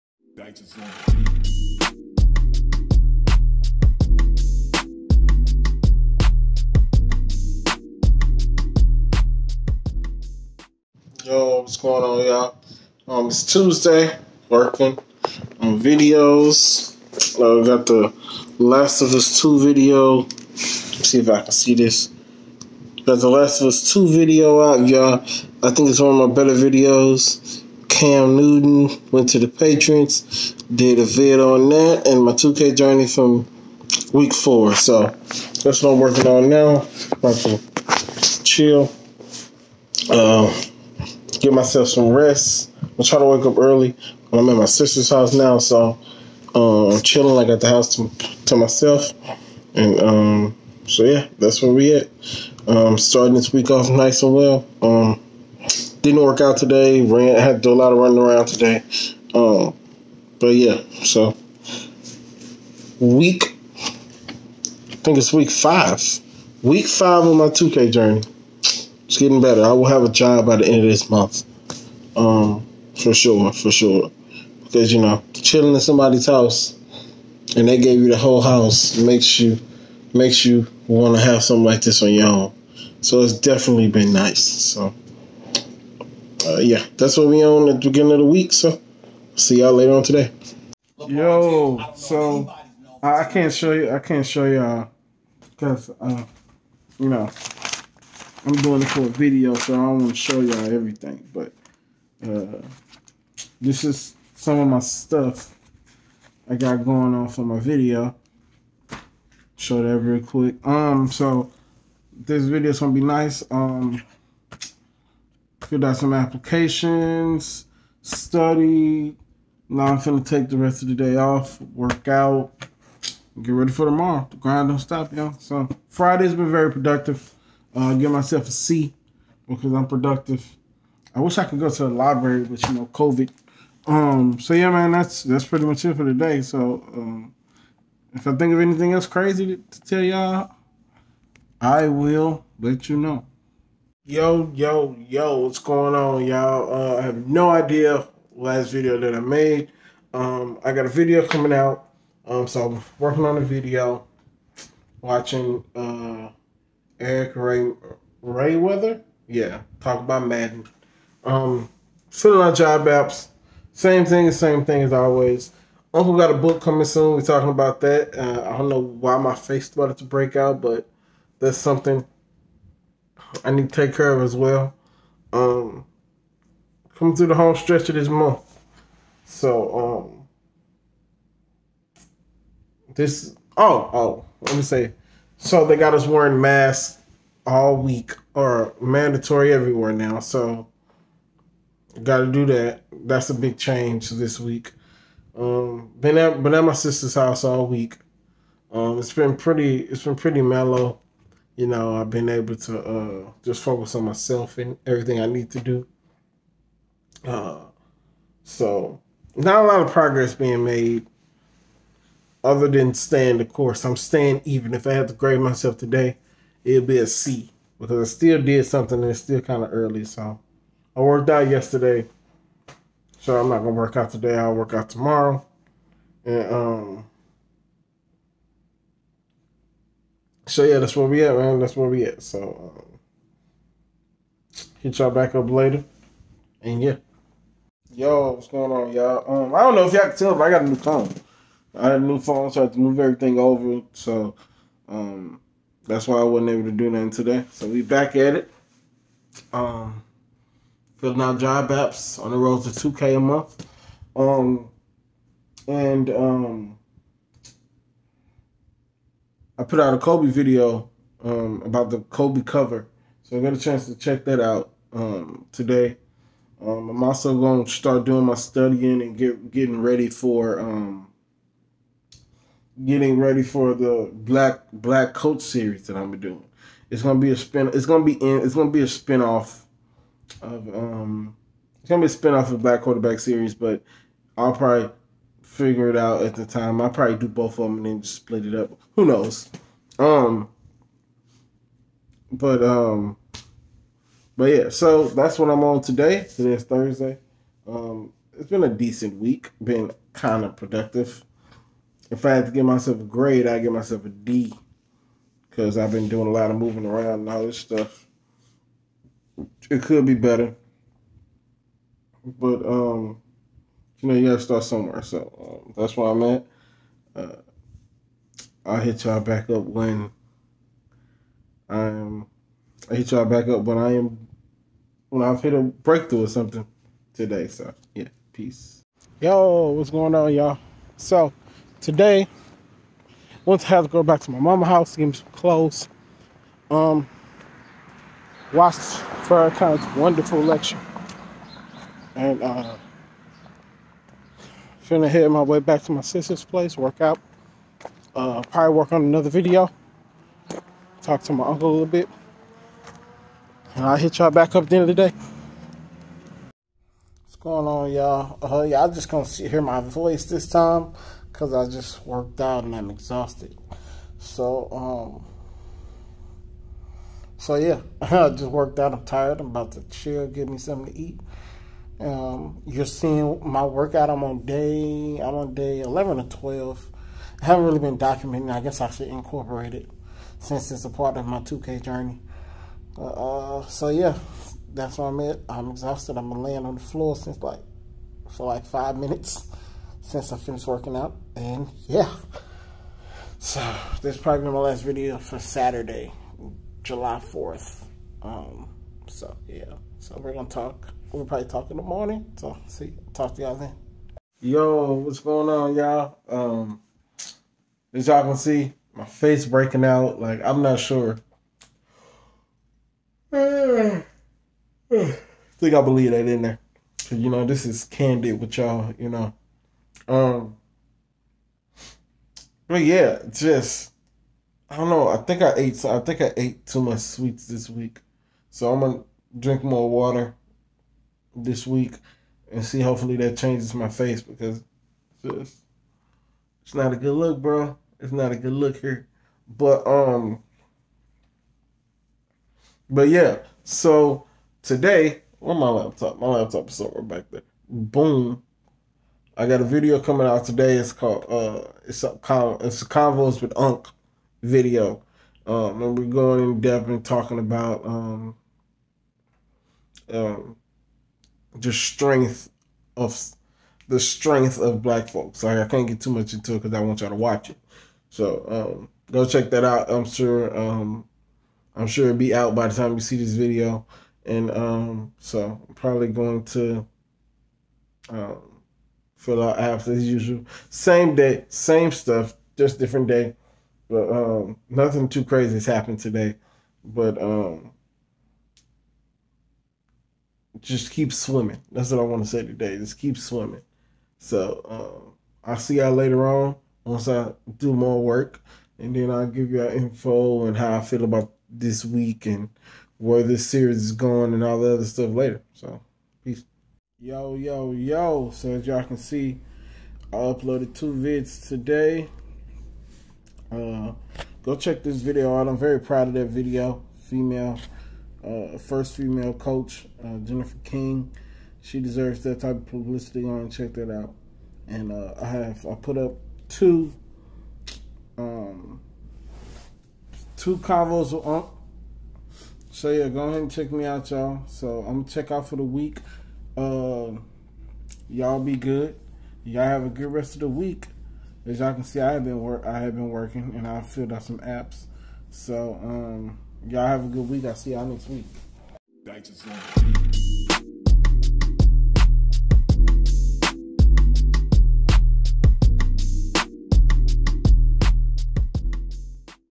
My 2k journey ..week 5
This is my video diary where i just talk about whatever i'm doing that day and how i'm doing on this road to making 2k a month doing what i love.